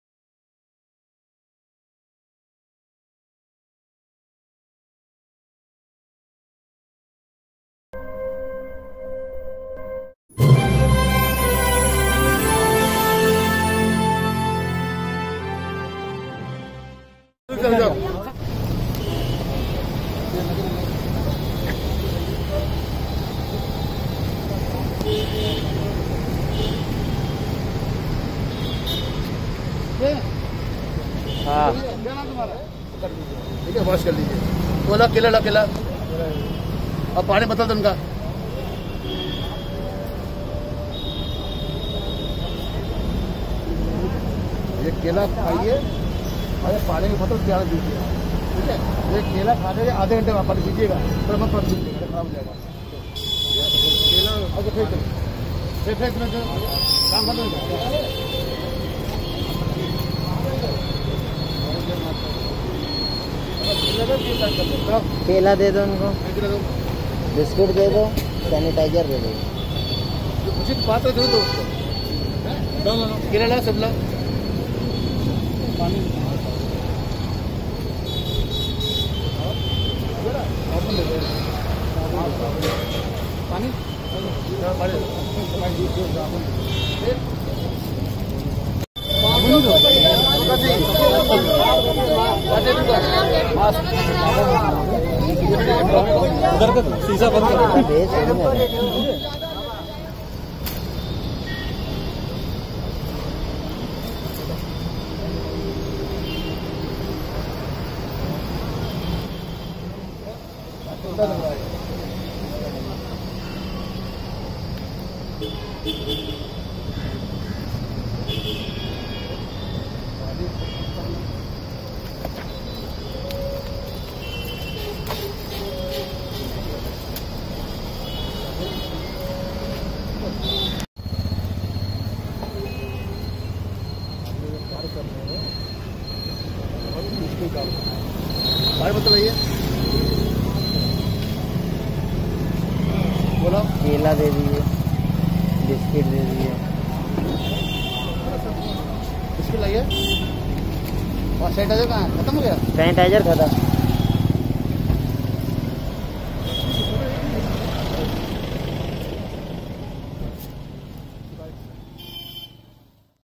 शौर्य फाउंडेशन ने बड़े मंगल पर शहर में बांटे फल,सेनिटाइजर एवं मास्क
फल,सैनिटाइजर मास्क,साबुन,कपड़े पानी की बोतल एवं खाद्य पदार्थ किया गया वितरण
हनुमान सेतु मंदिर से की गई शौर्य फाउंडेशन के अध्यक्ष अनुपम सिंह भंडारी द्वारा शुरूआत।
*विशेष संवाददाता ज़ाहिद अली की रिपोर्ट प्रेसमैन न्यूज लखनऊ  1 जून 2021 लखनऊ सिटी लाइव*